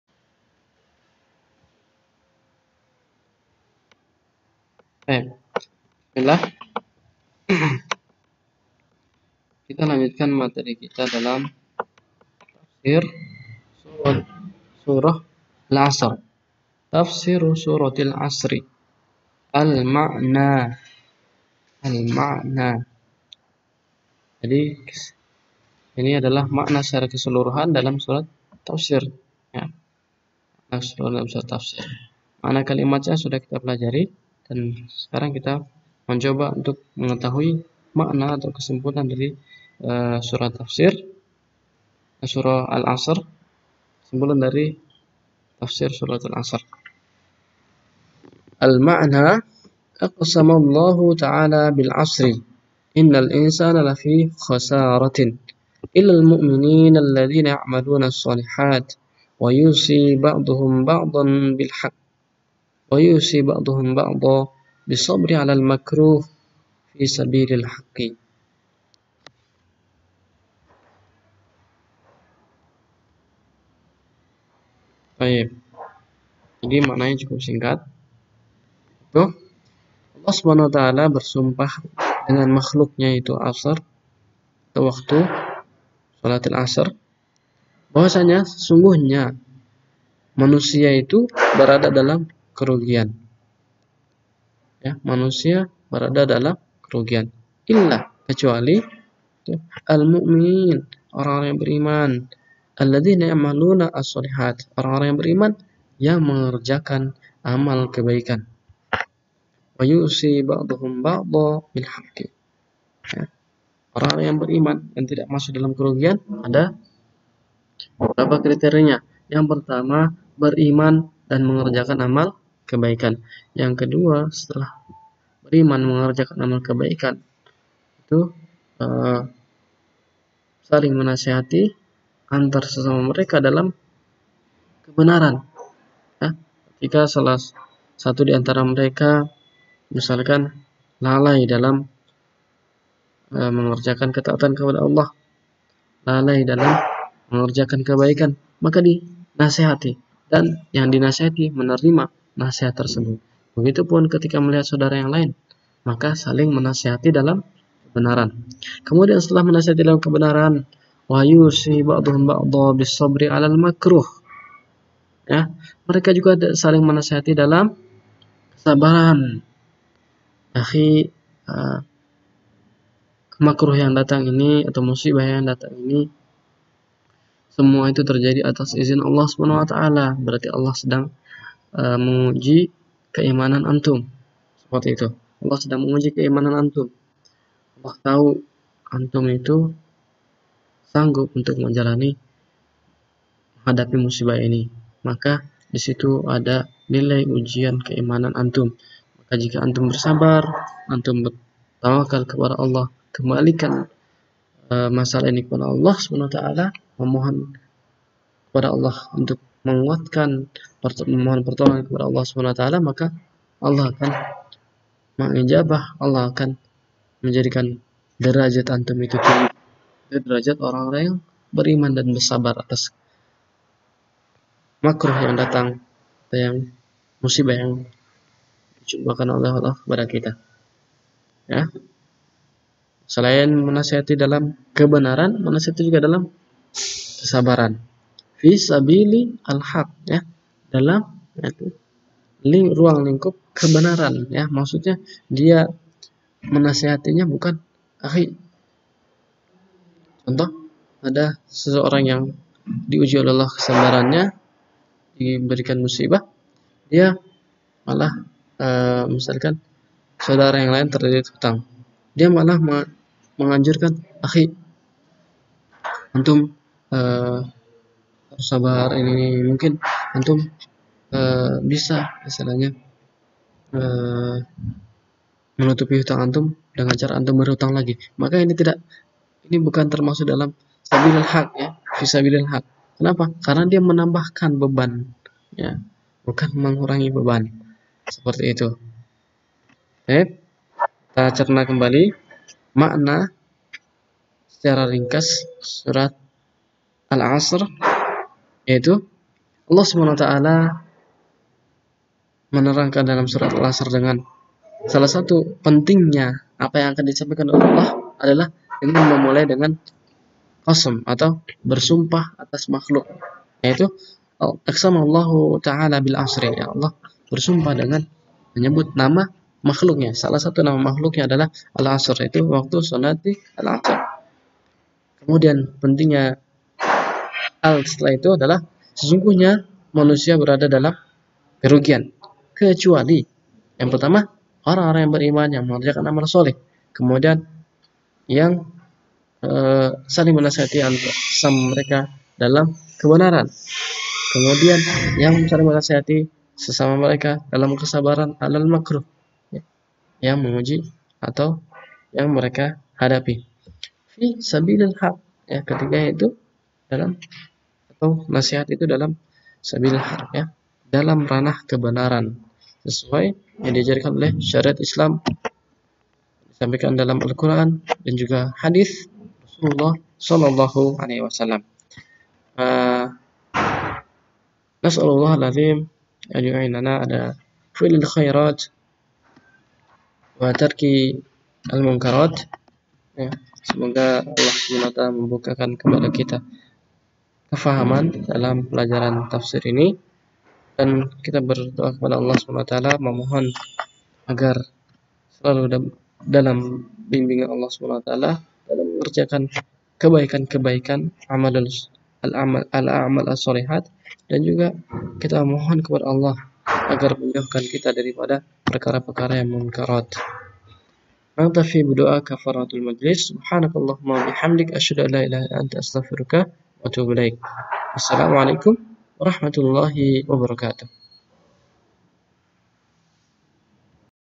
Tafsir Surah Al-'Ashr (part2)
Makna Singkat Tafsir surah Al-'Ashr
Kelas 7 Tahfidz Minhajul Haq